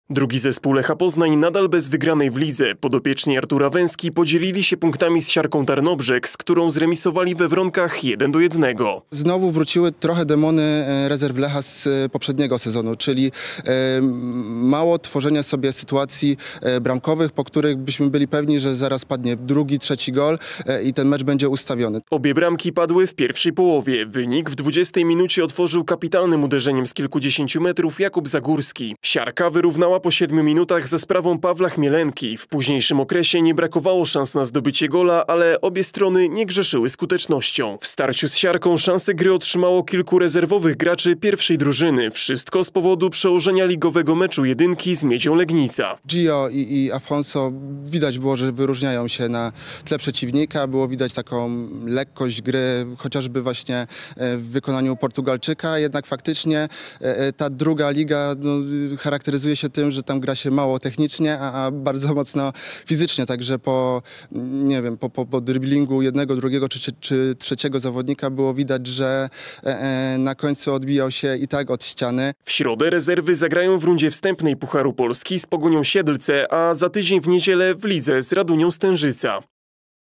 Rezerwy Lecha Poznań na remis z Siarką. Zagrali m. in. Sousa i Tsitaishvili
Link do artykułu W meczu II kolejki eWinner 2. ligi rezerwy Lecha Poznań podzieliły się punktami z Siarką Tarnobrzeg. W starciu z beniaminkiem rozgrywek ujrzeliśmy kilku graczy z pierwszej drużyny Kolejorza.